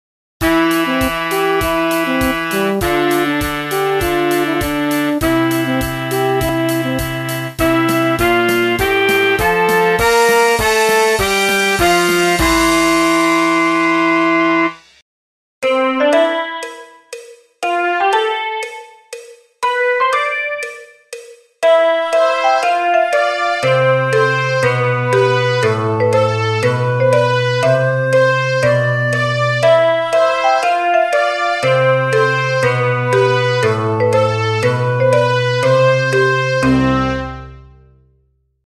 2 CGI Music Recreations
Decided to try something new and do some CGI Music recreations, there would've been a third one from Busy Bees but I wanna save that one for another time so you only get two. Feel free to use with credit.

P.S I used Flat.io for this!